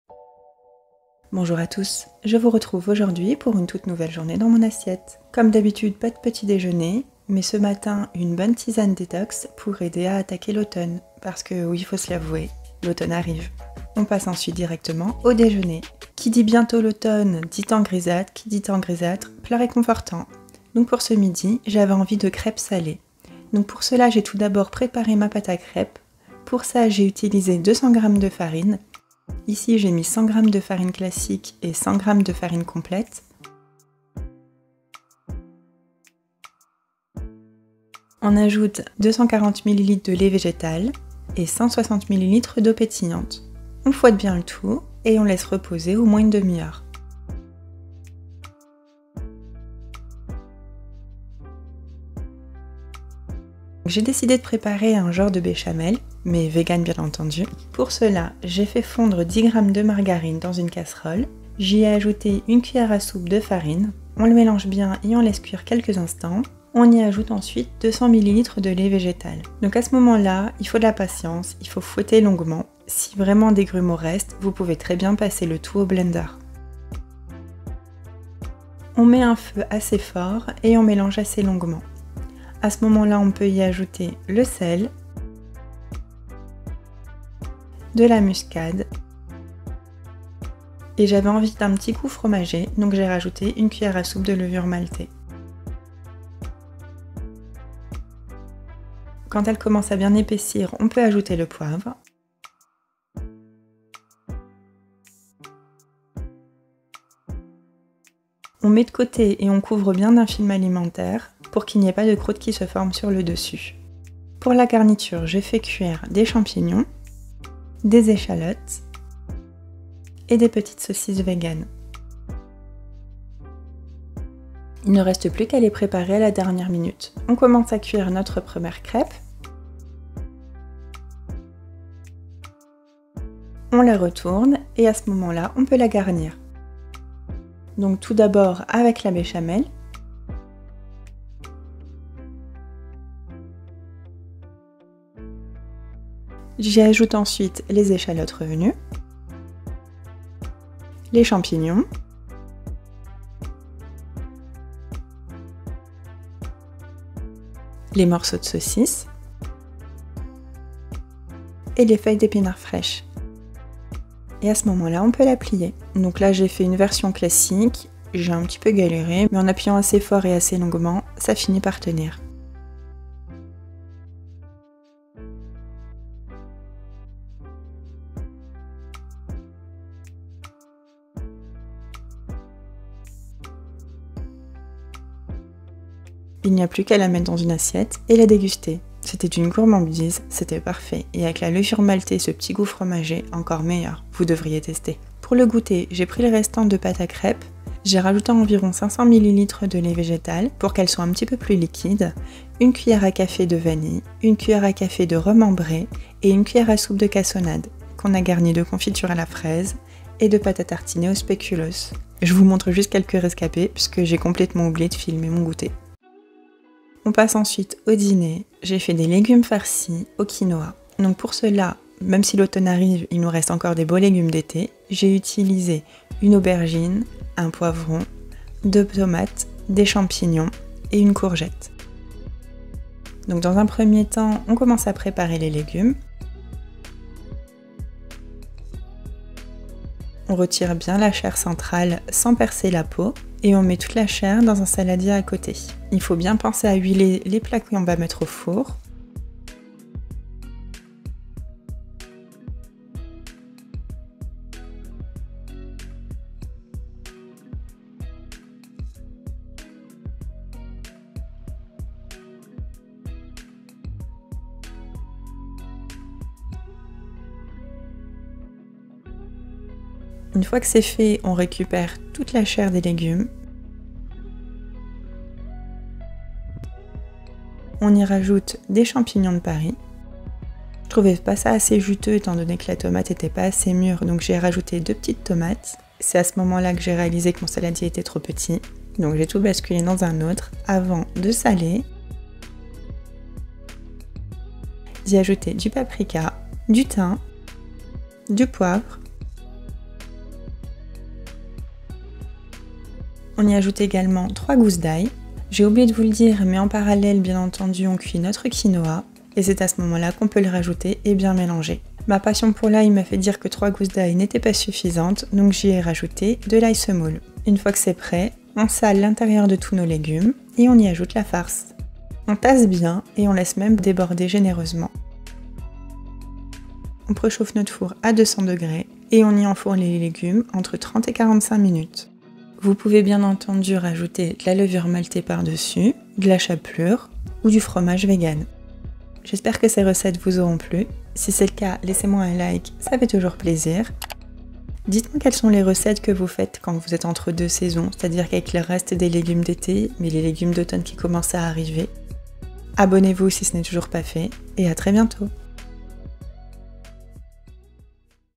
Une journée dans mon assiette | Vegan #15
Crêpes salées :
Pâte à crêpes 00:29
200 grammes de farine
½ cuillère à café de sel
240 millilitres de lait végétal
160 millilitres d’eau pétillante

Béchamel 00:53
10 grammes de margarine
1 cuillère à soupe de farine
200 millilitres de lait végétal
Sel
Muscade
1 cuillère à soupe de levure maltée
Poivre

Garniture 01:54
Champignons
Echalotes
Epinards
Saucisse végane

Crêpes sucrées 01:16
La base salée additionnée de :
50 millilitres de lait végétal
1 cuillère à café de vanille
1 cuillère a café de rhum ambré
1 cuillère à soupe de cassonade

Légumes farcis 03:38
150 grammes de quinoa cru
2 tomates à farcir
1 aubergine
1 courgette
1 poivron rouge
3 gousses d’ail
Sel
Poivre
Paprika
Thym
Ail semoule
45 minutes à 200 °